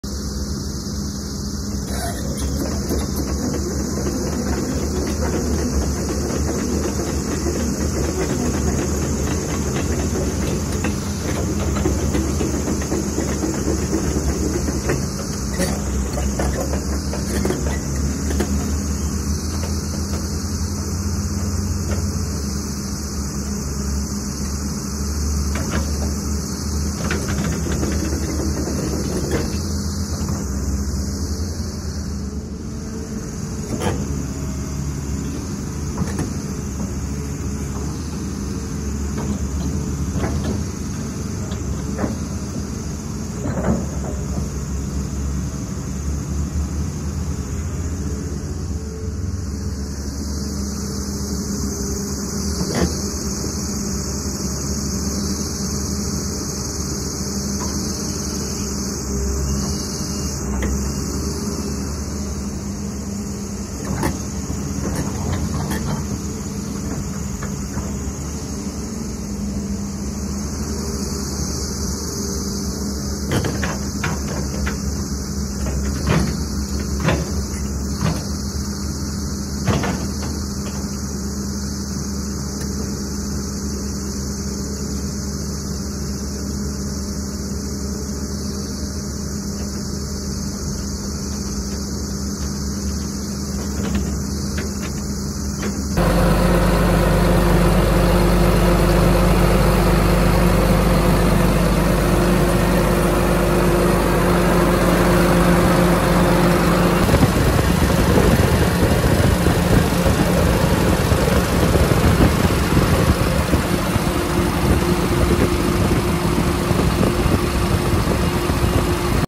2010 Caterpillar 320D Hydraulic Excavator - Dubai, UAE Auction | 16 & 17 June
NOTE: This auction will be Timed Auction (TAL) only - no live auction
Online bidding will open on June 10th at 9AM!
Viewing days: 10 -15 June